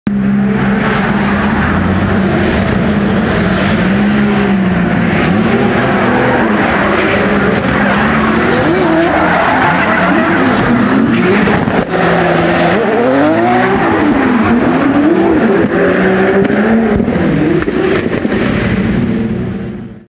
Jestli byla RIGA rychlá trať? Tady je odpověď a jízda s Piotrem Wieckem
Driftmasters King of RIGA 2020